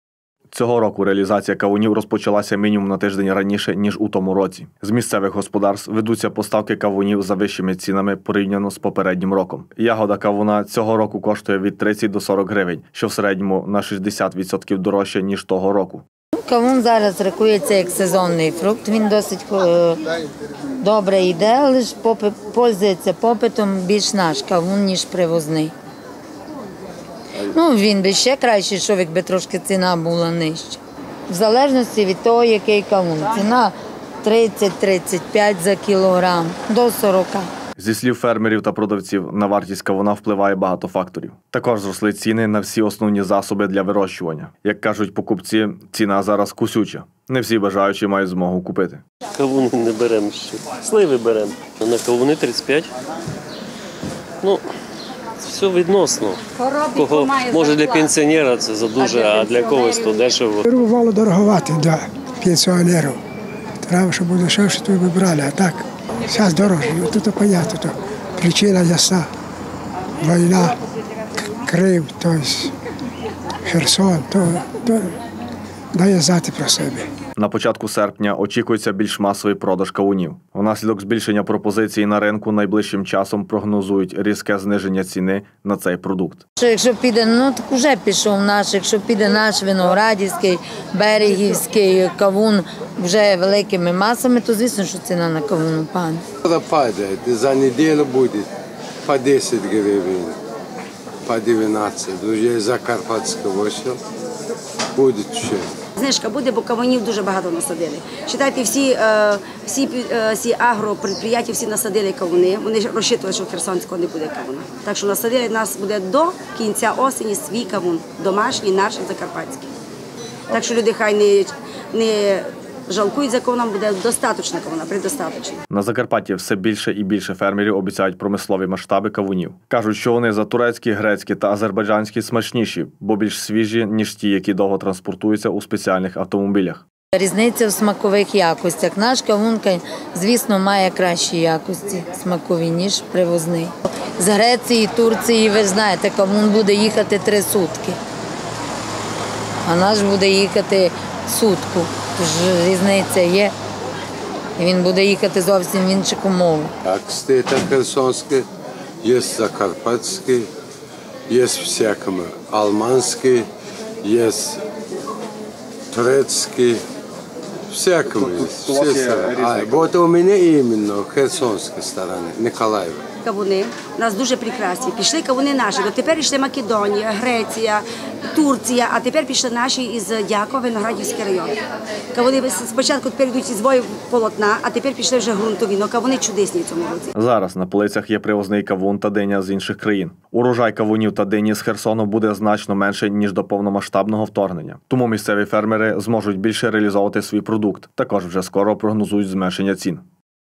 Вартість кавуна
Цього року реалізація кавунів розпочалася мінімум на тиждень раніше, ніж у тому році. З місцевих господарств ведуться поставки кавунів за вищими цінами порівняно  з попереднім роком. Ягода кавуна цього року коштує від 30 до 40 гривень, що в середньому на 60 відсотків дорожче, ніж того року. Зі слів фермерів та продавців на вартість кавуна впливає багато факторів. Також зросли ціни на всі основні засоби для вирощування. Як кажуть покупці, ціна зараз кусюча, не всі бажаючі  мають змогу купити. На початку серпня очікується більш масовий продаж кавунів. Внаслідок збільшення пропозиції на ринку, найближчим часом прогнозують різке зниження ціни на цей продукт. На Закарпатті все більше і більше фермерів обіцяють промислові масштаби кавунів. Кажуть, що вони за турецькі, грецькі та азербайджанські смачніші, бо більш свіжі, ніж ті, які довго транспортуються у спеціальних автомобілях. Зараз на полицях є привозний кавун та диня  з інших країн. Урожай кавунів та дині з Херсону буде значно менший ніж до повномасштабного вторгнення. Тому місцеві фермери зможуть більше реалізовувати свій продукт, також вже скоро прогнозують зменшення цін.